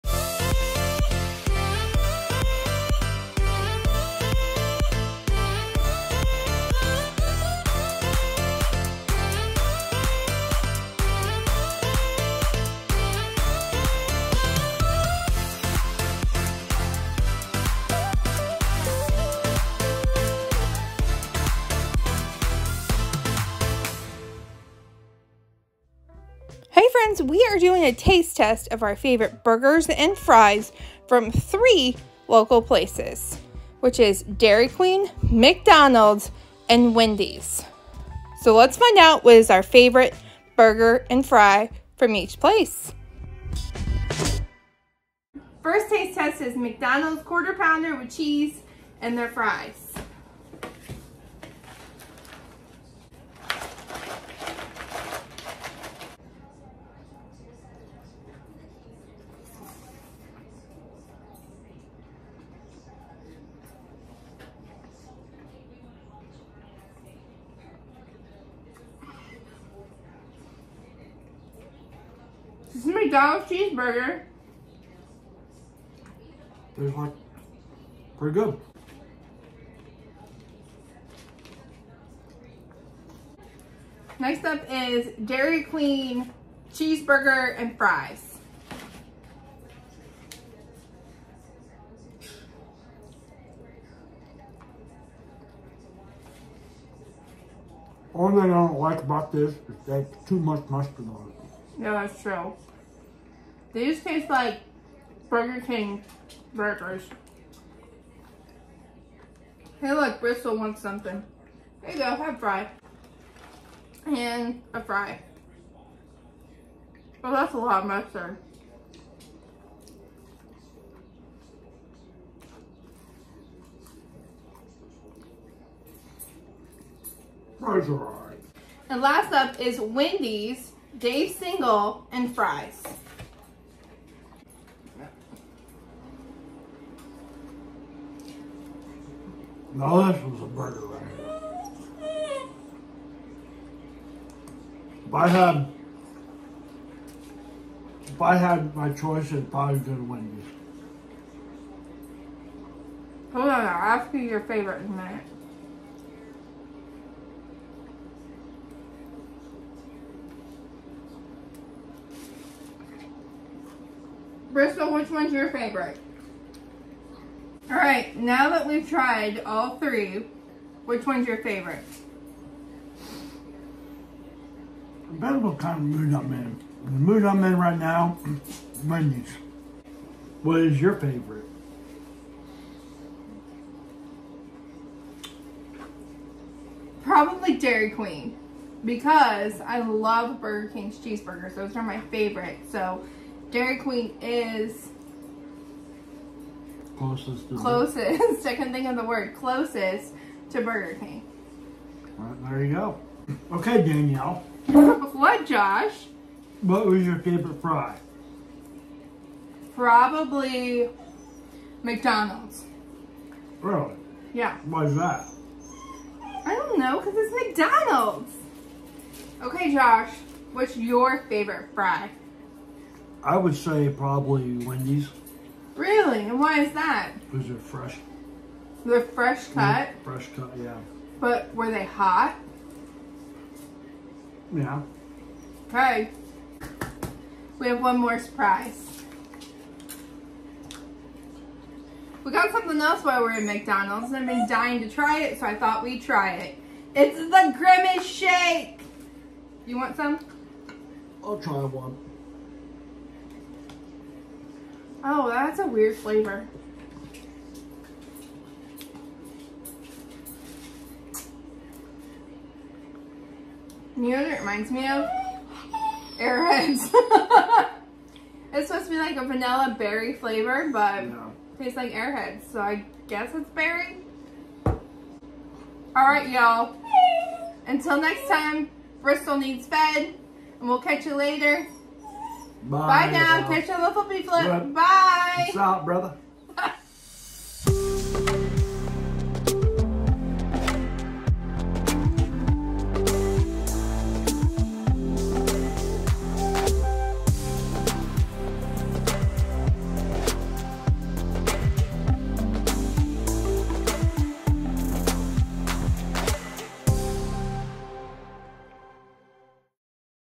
Taste Test: Who Has The Best Burger and Fries.
In today’a blog we do a taste test of the 3 fast food places in our town. McDonald’s, Wendy’s and Dairy Queen. Let’s see who has the best burger and fries!